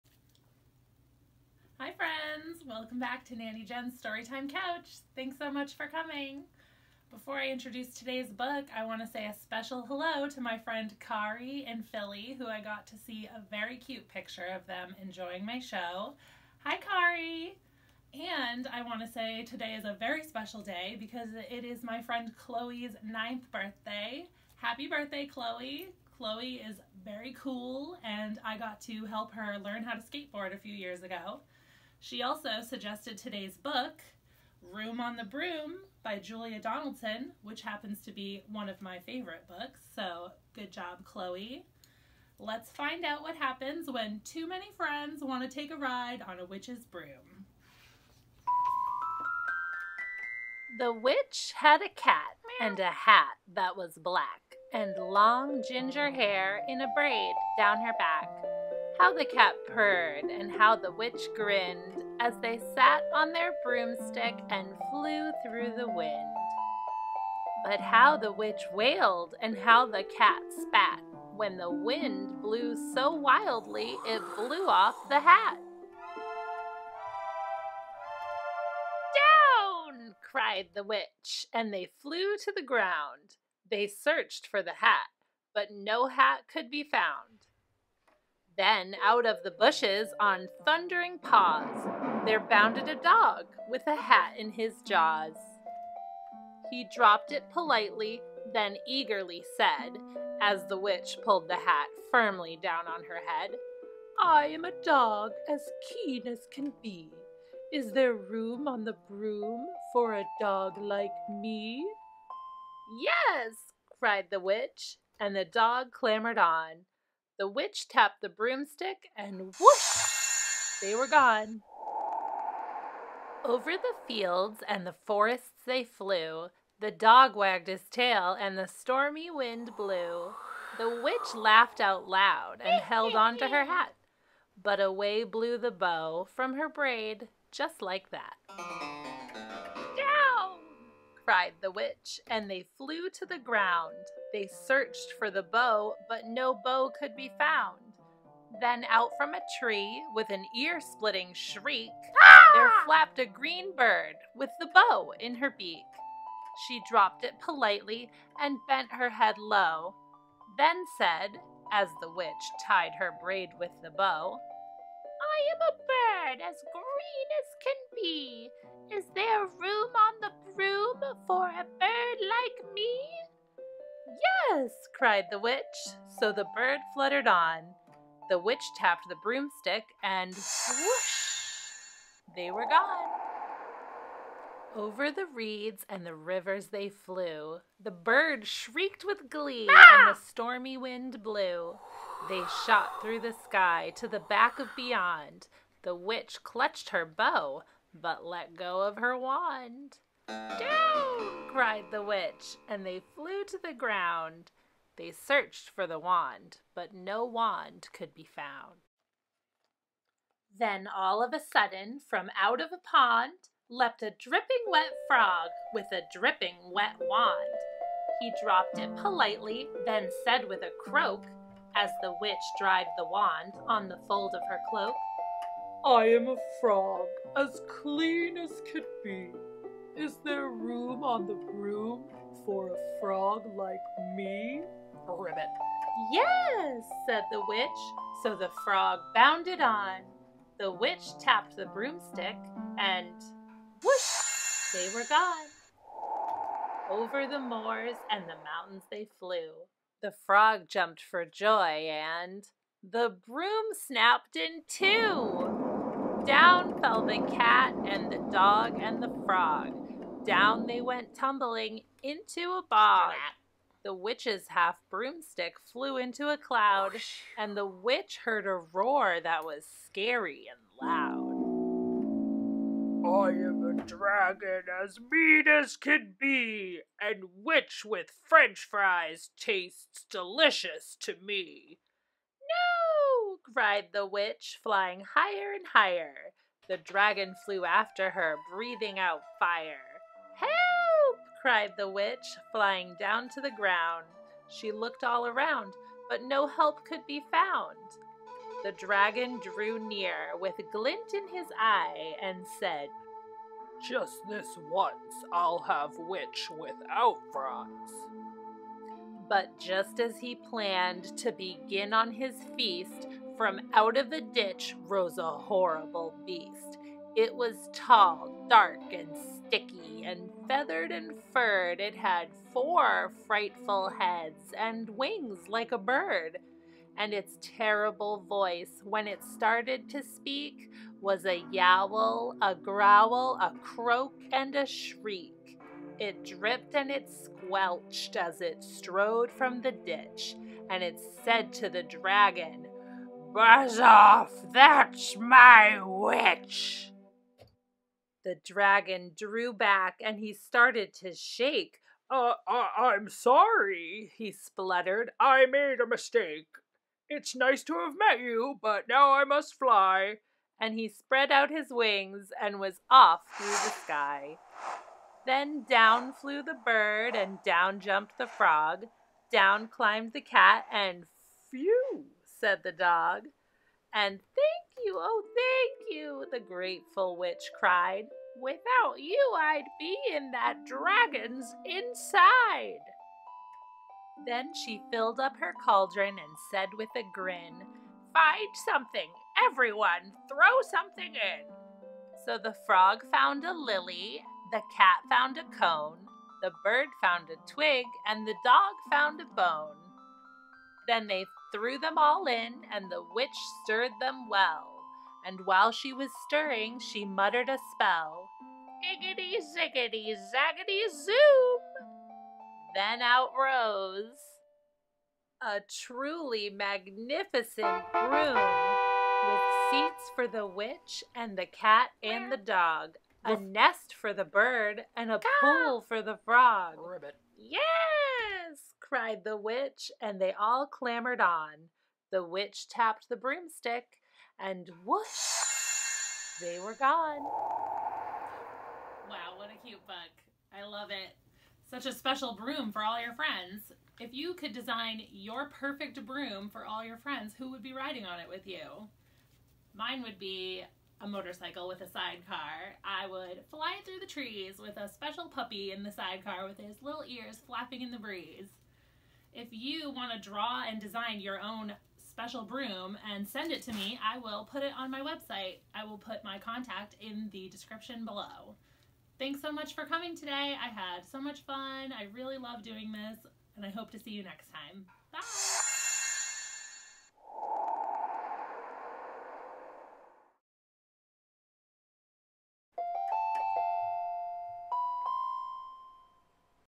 Nanny Jen Story Time Eps. 5- Room On The Broom
Nanny Jen Reads "Room On The Broom" By Julia Donaldson and Axel Scheffler Penguin Books 2001. In this story too many friends want a ride on this witches broom! find out what happens when the broom can't work it's magic anymore!